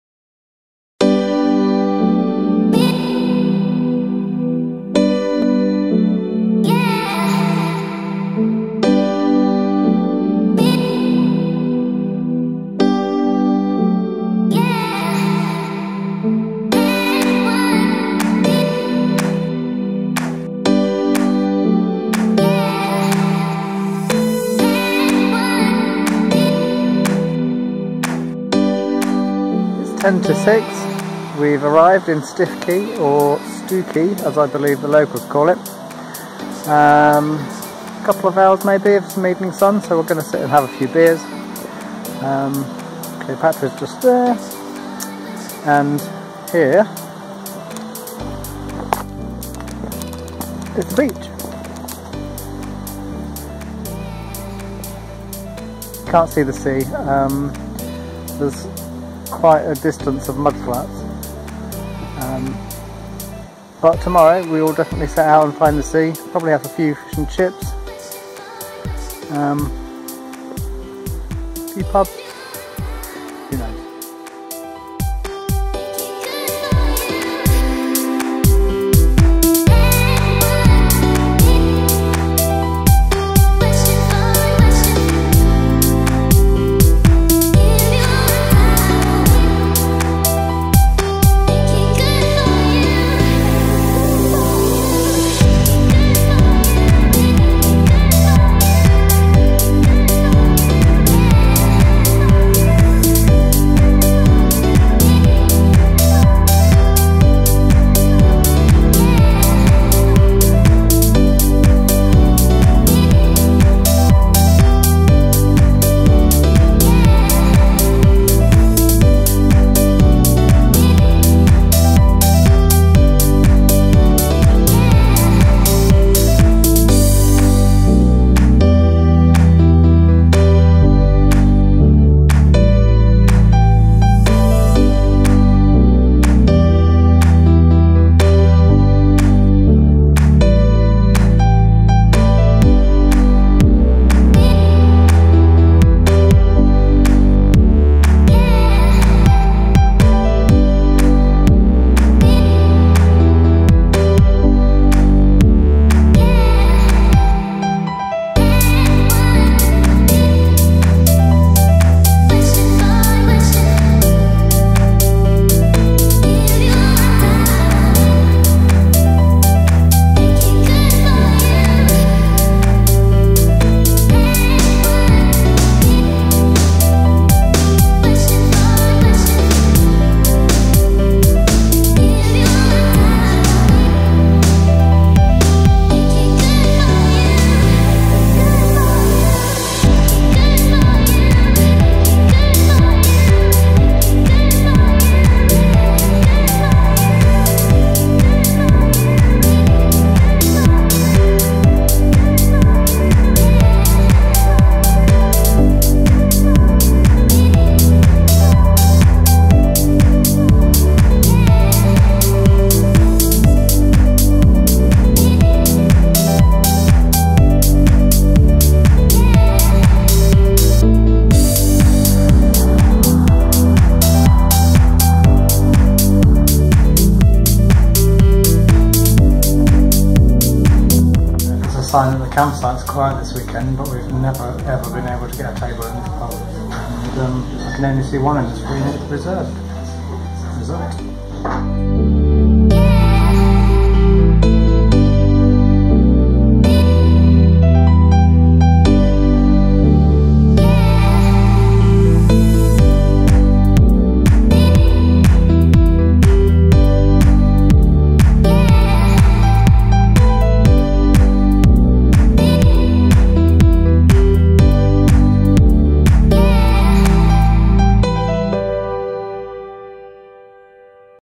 Stiffkey camping and Wells-next-the-Sea
Cleopatra the Campervan spends a weekend at High Sands Creek campsite in Stiffkey on the north Norfolk coast, and Mark and Tony walk along the Norfolk Coastal Path to visit Wells-next-the-Sea.